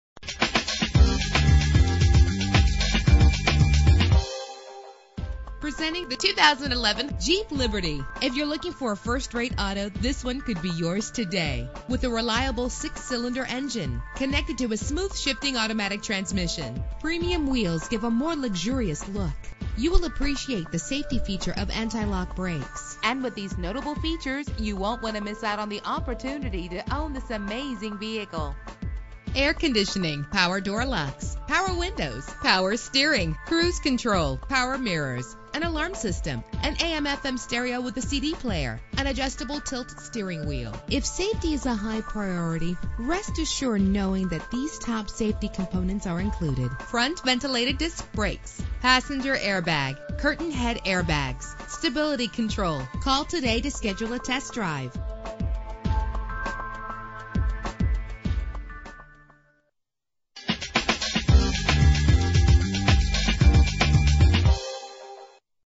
2011 Jeep Liberty - Little Rock AR
Year: 2011
Make: Jeep
Model: Liberty
Trim: Sport
Engine: 3.7 liter 6 cylinder 12 valve
Transmission: 4-Speed Automatic
Color: Bright White Clearcoat
Mileage:
Address:
4600 South University Ave